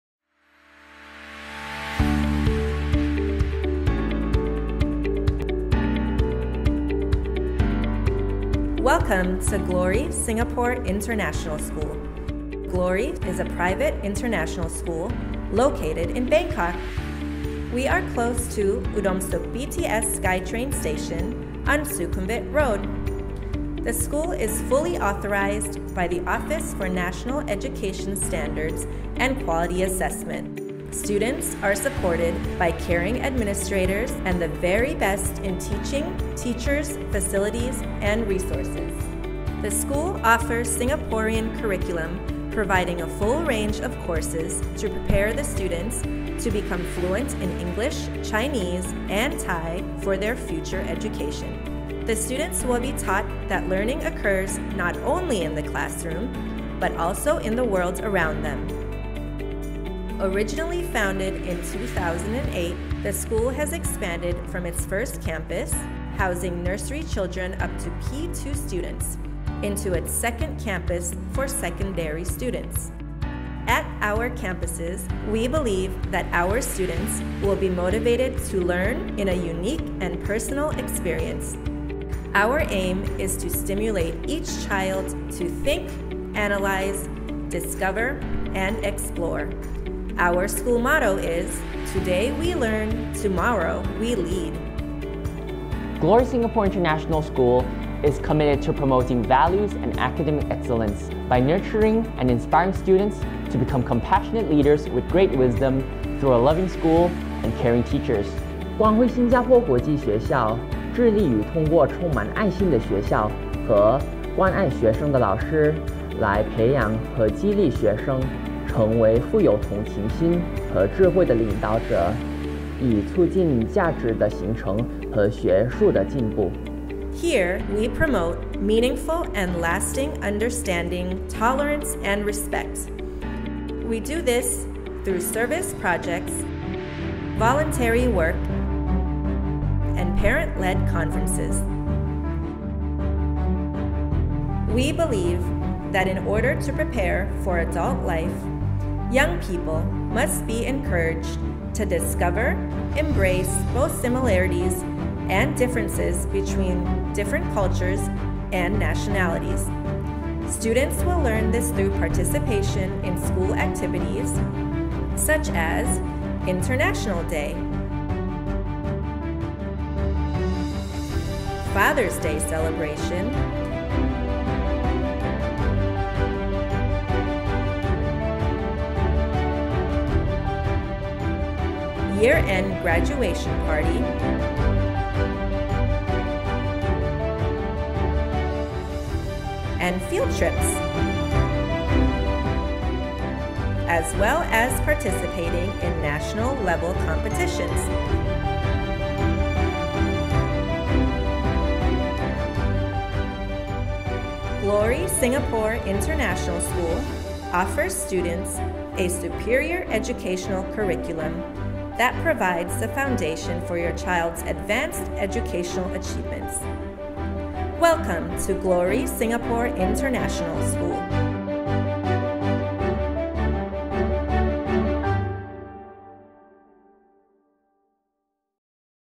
Welcome to Glory Singapore International School
GSIS is a private international school, offering a program for Nursery through Grade 12 based on Singapore and British Curriculums. Founded in 2008 with modest beginning, Glory has been offering Trilingual language classes (English, Thai, and Chinese) with the emphasis on both academic and personal development.